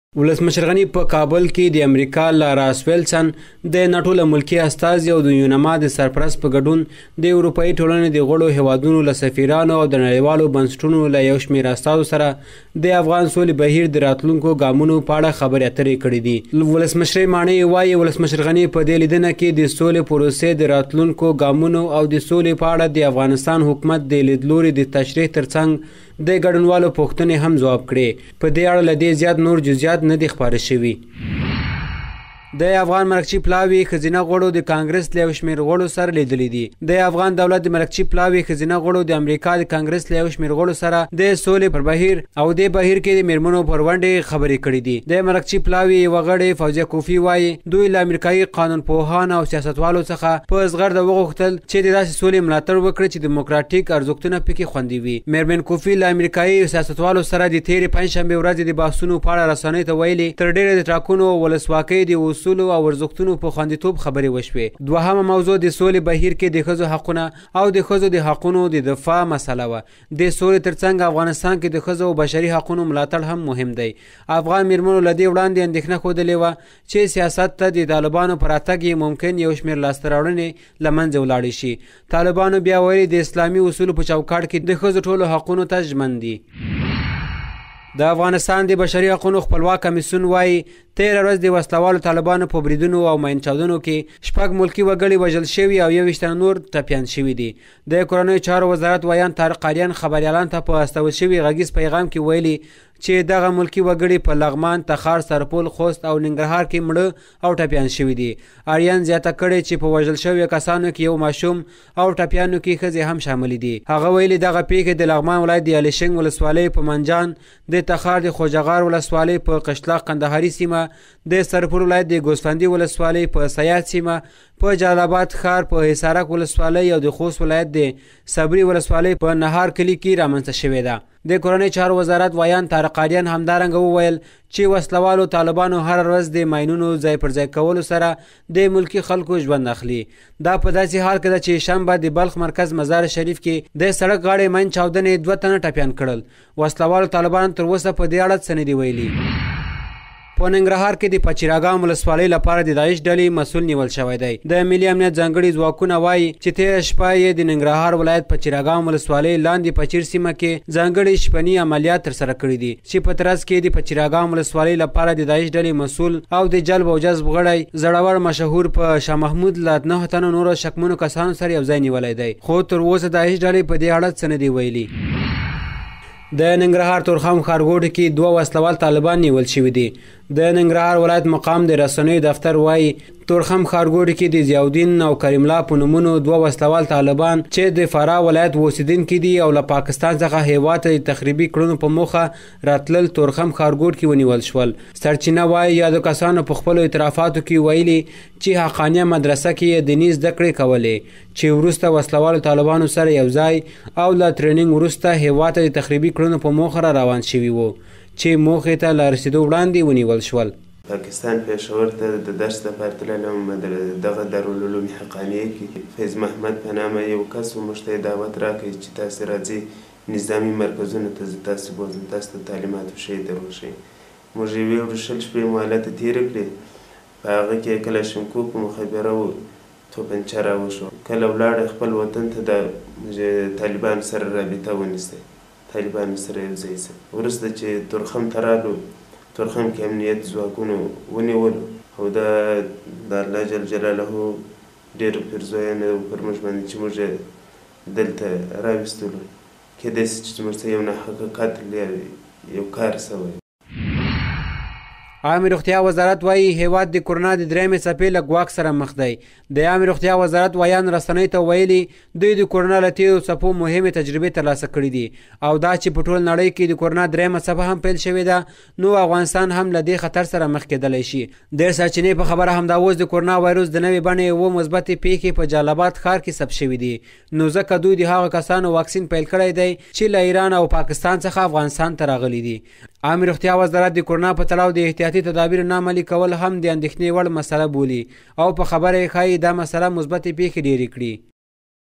دلته هم د نن ورځي لنډ او مهم خبرونه لیدلي او اوریدلي شئ
۱:ولسمشر غني په کابل کې د امریکا له شارژدافیر، د ناټو له ملکي استازي او د یونما د سرپرست په ګډون د اروپایي ټولنې د غړو هېوادونو له سفیرانو او د نړیوالو بنسټونو له یو شمېر استازو سره د افغان سولې بهیر د راتلونکو ګامونو په اړه خبرې اترې کړې دي
۲:ننګرهار کې د پچیر اګام ولسوالۍ لپاره د داعش ډلې مسوول نیول شوی
۳:د افغان مرکچي پلاوي ښځینه غړو د کانګرس له یو شمېر غړو سره لیدلي
۴:د افغانستان د بشري حقونو خپلواک کمېسیون وايي، تیره ورځ د وسله والو طالبانو په بریدونو او ماین چاودنو کې شپږ ملکي وګړي وژل شوي او ۲۱ تنه مړه شوي دي
۵:عامې روغتیا وزارت وایي هېواد د کرونا د درېیمې څپې له ګواښ سره مخ دی.
۶:د ننګرهار تورخم ښارګوټي کې دوه وسله وال طالبان ونيول شوي